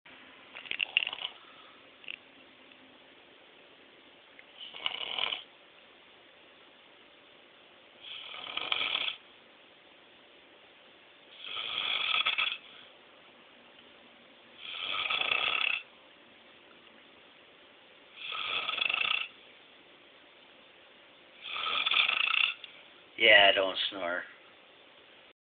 Dave Don't Snore
Dave says that he does not snore. Well we are out of town working and I'm bunking with Dave.